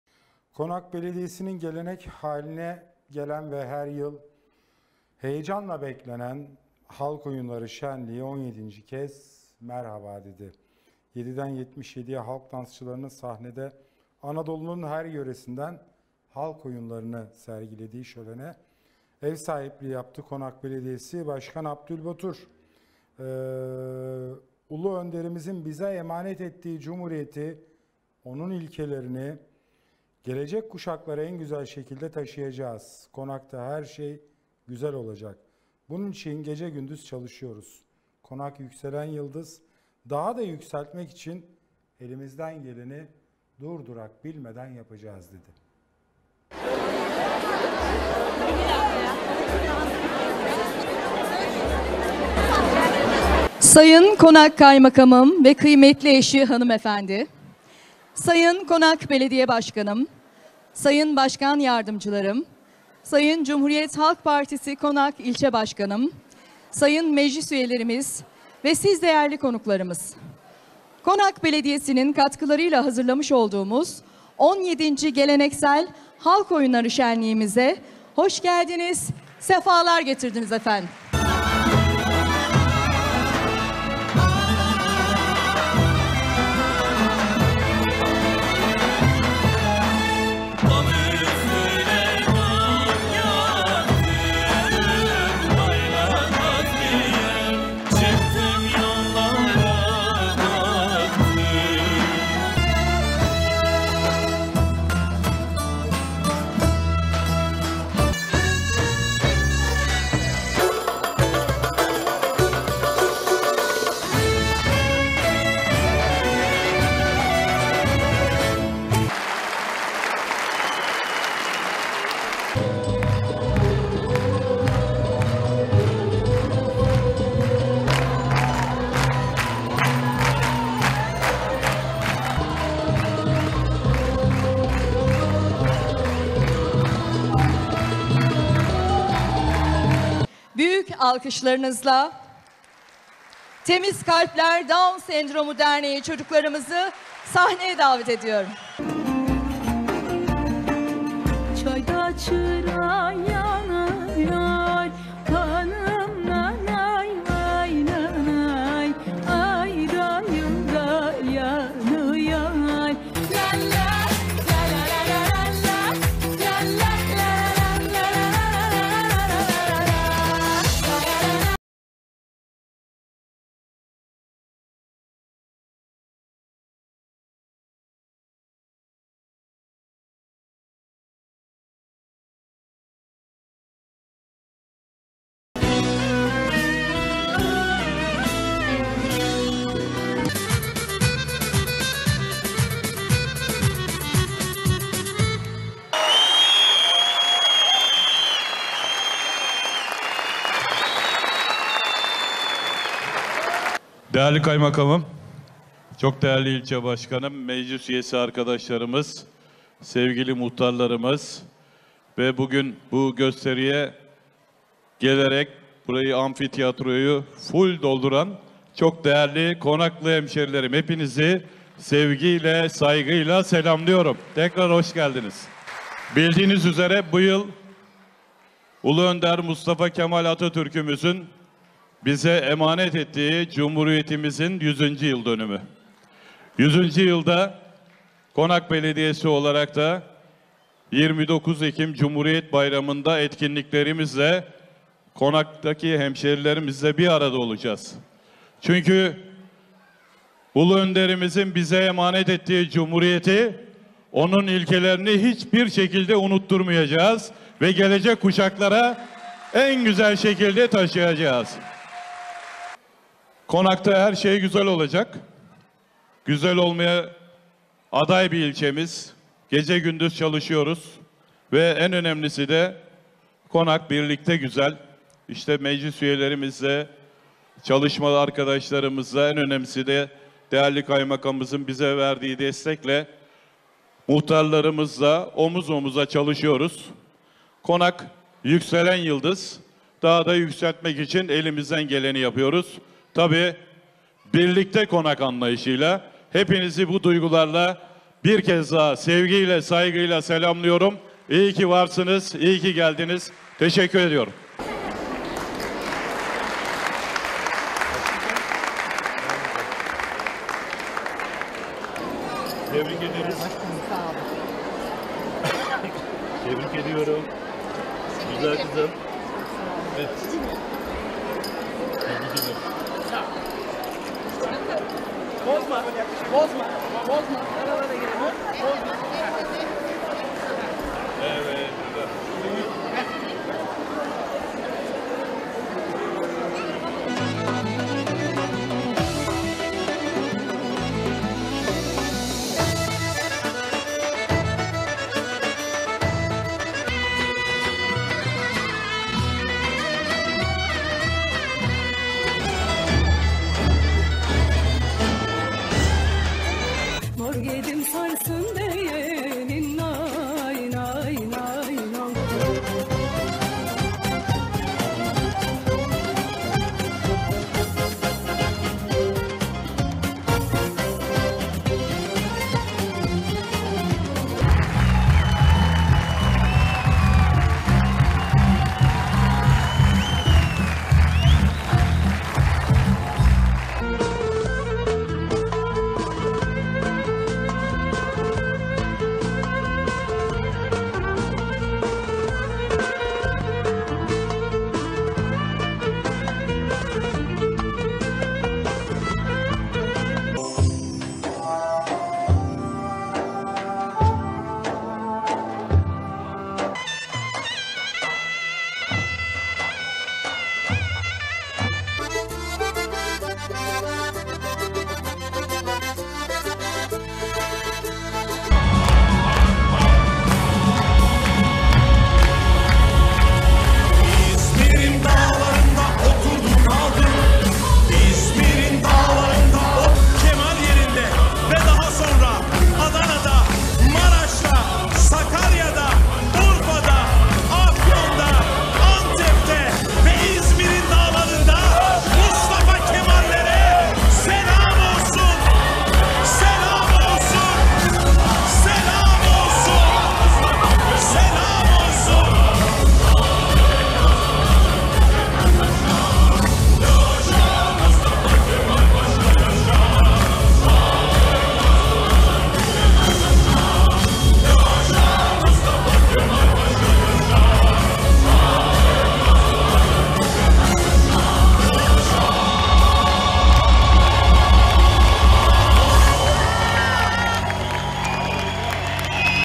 Konak’ta 7’den 77’ye halk oyunları şenliği
Konak Belediyesi’nin bu yıl 17’ncisini düzenlediği Halk Oyunları Şenliği’nde konuşan Başkan Batur, Anadolu’nun her yöresinden halk oyunları sergileyen ekipleri kutladı ve “Çünkü Ulu Önderimizin bize emanet ettiği cumhuriyeti, O’nun ilkelerini gelecek kuşaklara en güzel şekilde taşıyacağız. Konak’ta her şey güzel olacak. Bunun için gece gündüz çalışıyoruz Konak, yükselen yıldız… Daha da yükseltmek için elimizden geleni yapıyoruz” dedi.